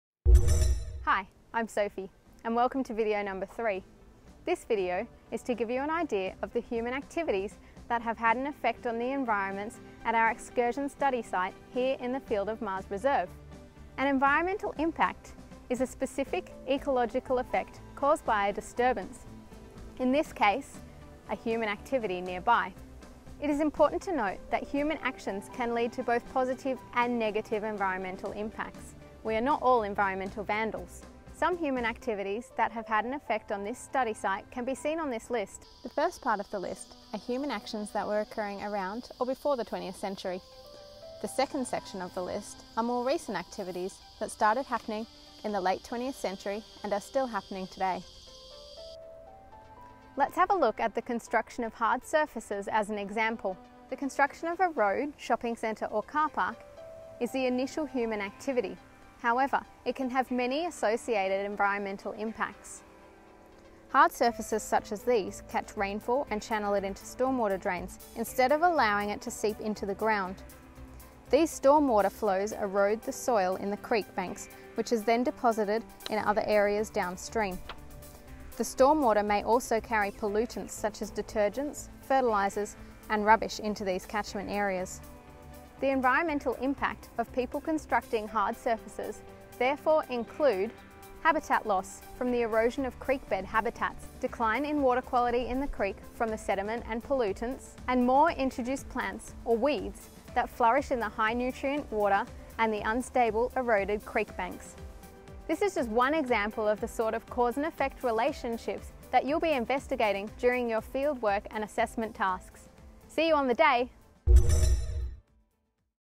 EES fieldwork video 3: Human impacts
This is the third video in a series designed to help Earth and Environmental science students as they prepare for their fieldwork excursion for the "Human impacts" topic.